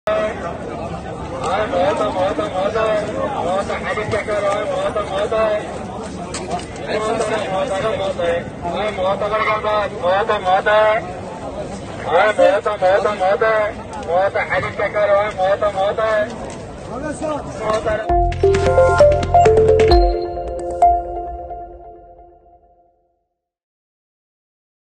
مشهد يحصل لاول مرة، بائع الايسكريم يتجول في المنطقة الخضراء
مشهد يحصل لاول مرة، بائع_الايسكريم يتجول في المنطقة_الخضراء بعد اقتحامها